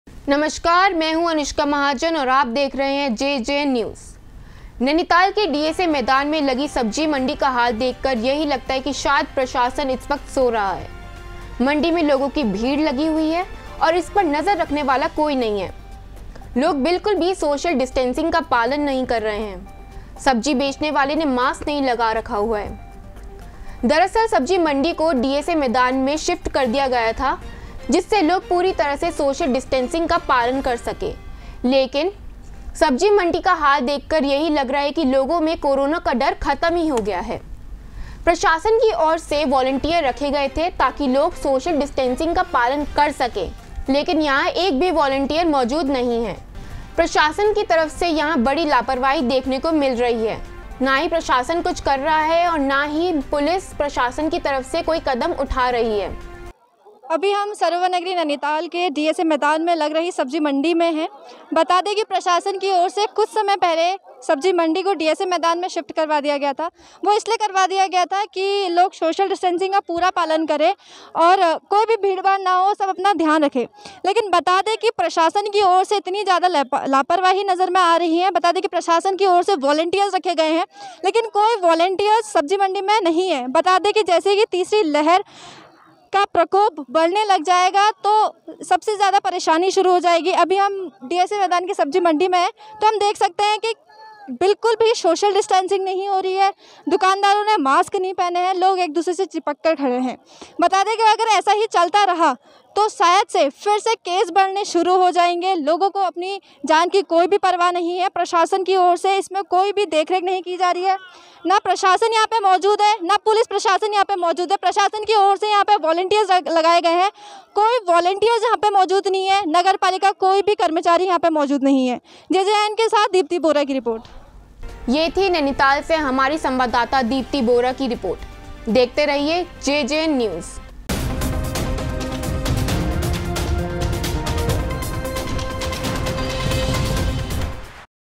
नैनीताल में लगी सब्जी मंडी का बुरा हाल, नही हो रहा कोरोना नियमों का पालन ||JJN News
Research/Script - Nidhi Bisht
Anchor - Anushka Mahajan
Editor - Priya Bhouriyal

JJN News Nainital,
JJN News,